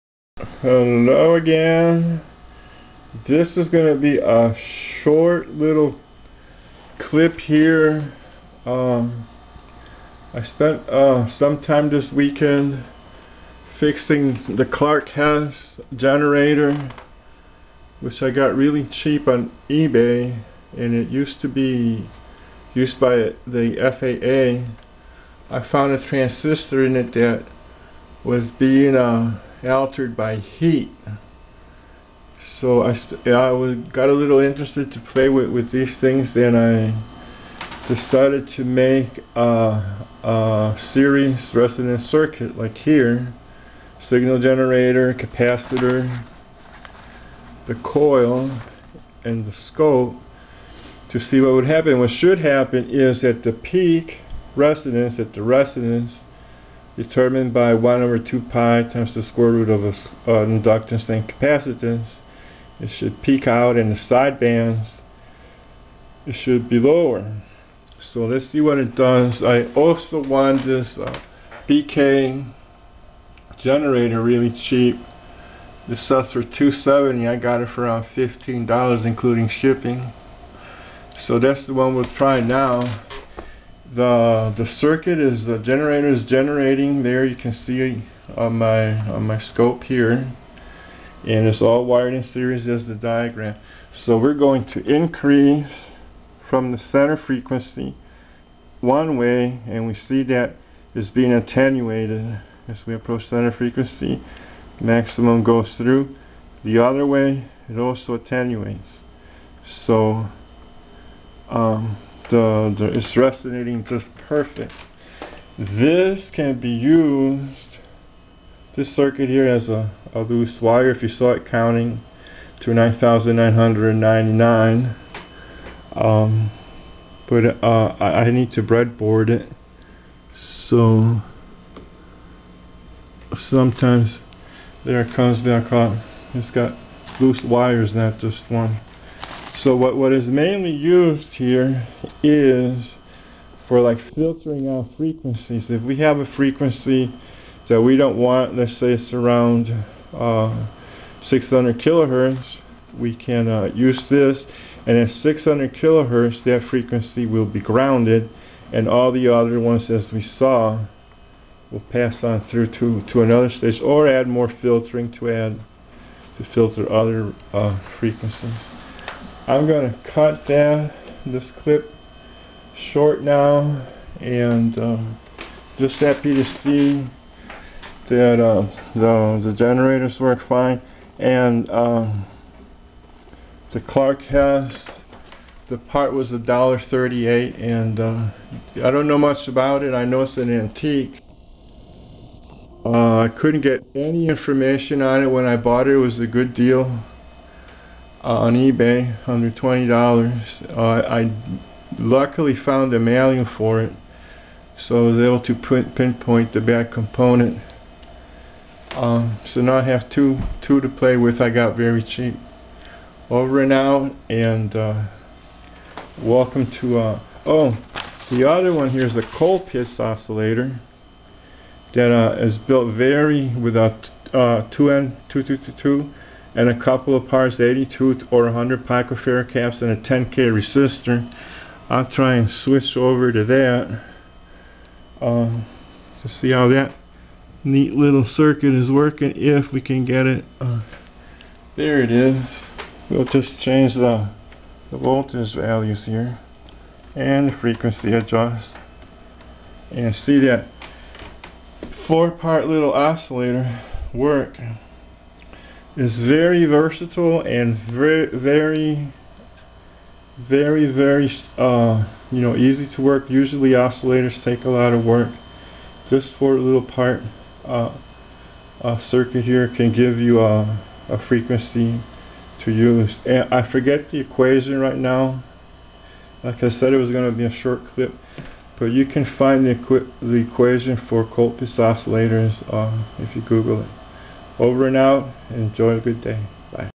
series resonance.mov
simple series resonant circuit. For the colpitts oscillator the transistor used is a 2N2222 found at radio shack, for ten cents a piece.

the inductor is 330 micro-Henrie  but with the 82pf (100 pf) capacitors i got it working with over 1 Milli-Henrie coil. so if you wind a lot of turns around a nail you might get it to work just as well.